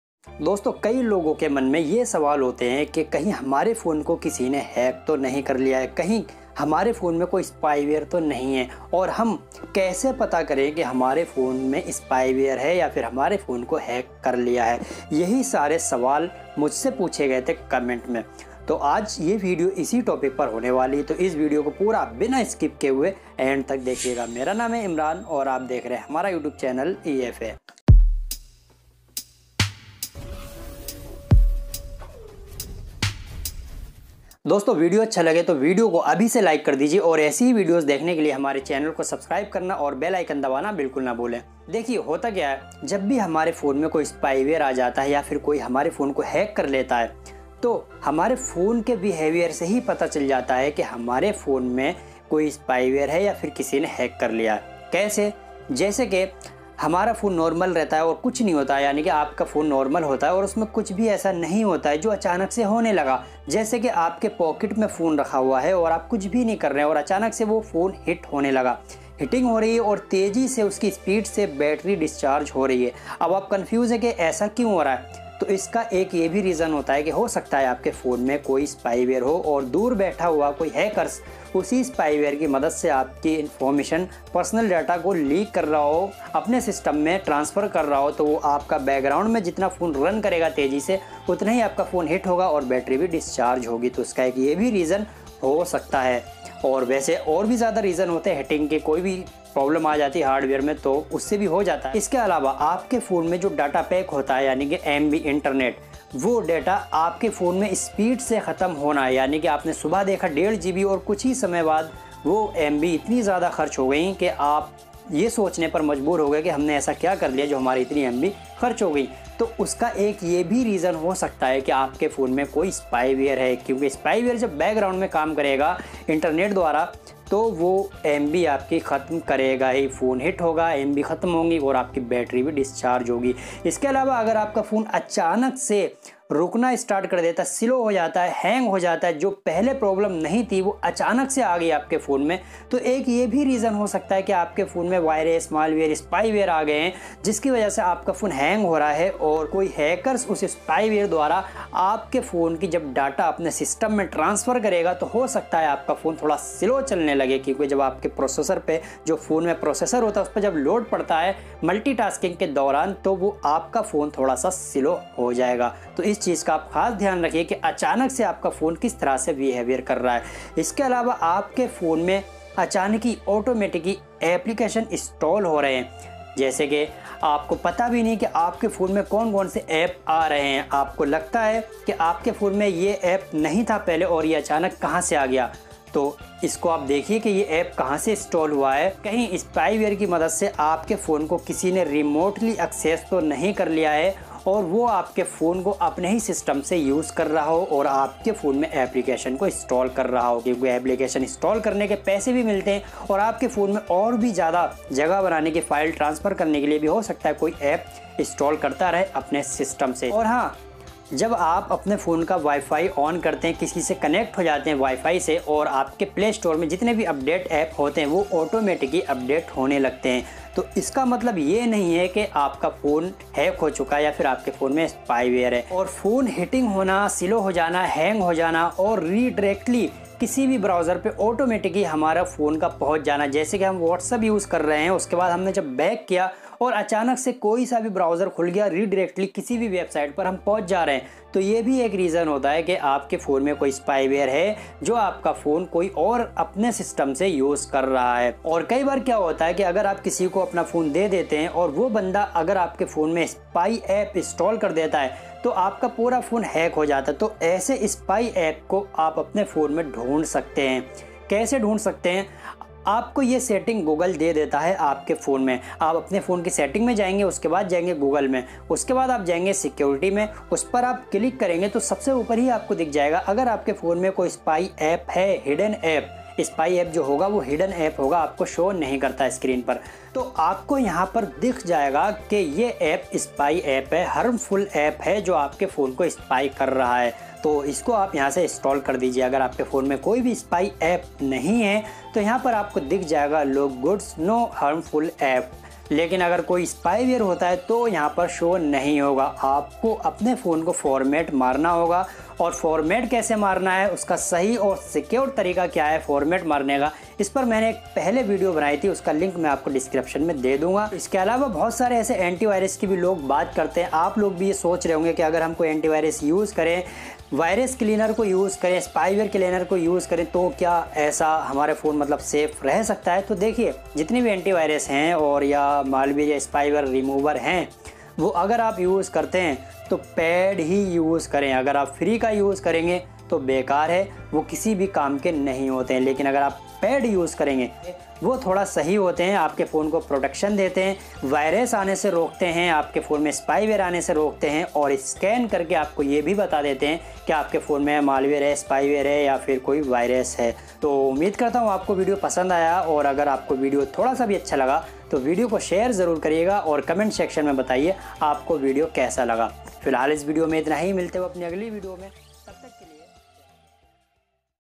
आपके Android Mobile में Spyware/Virus है कैसे पता करें | How To Find Spyware In Your Android Mobile
The purpose of this video is only education. In this video I have told how to detect if our phone has malware or spyware and how to protect the phone from malware. Keep Safe Your Phone...

By Everything For All